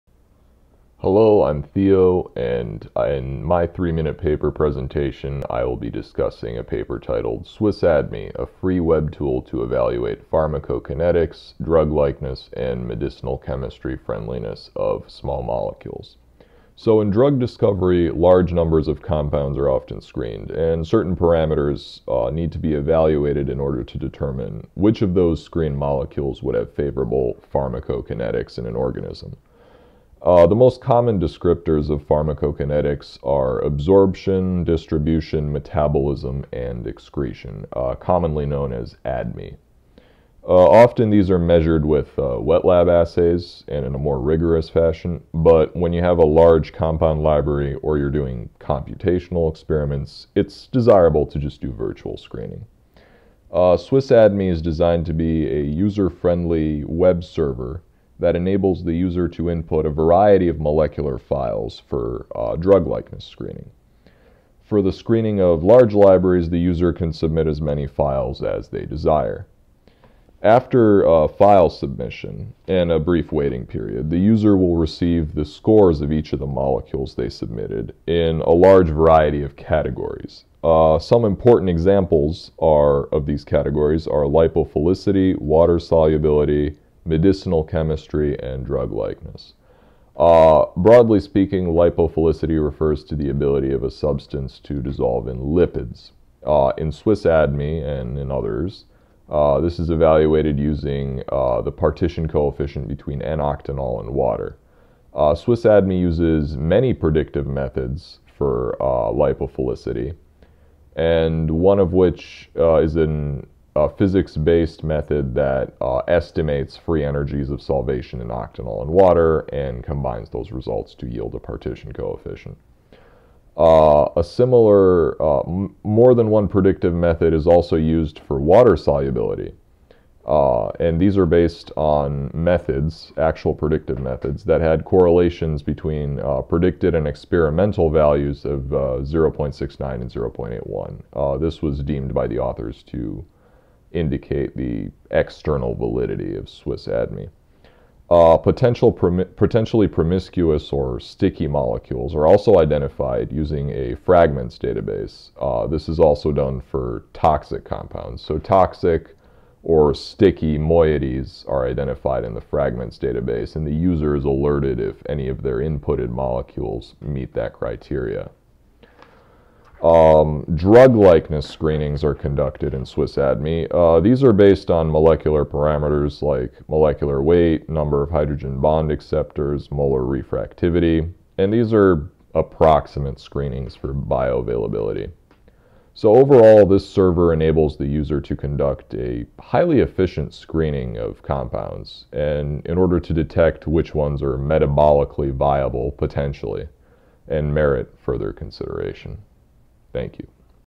SwissADME overview; Zoete et al (2017).
Presenter: Theodore Belecciu
Daina, A., Michielin, O., & Zoete, V. (2017). SwissADME: a free web tool to evaluate pharmacokinetics, drug-likeness and medicinal chemistry friendliness of small molecules. Scientific Reports, 7(1), 1–13.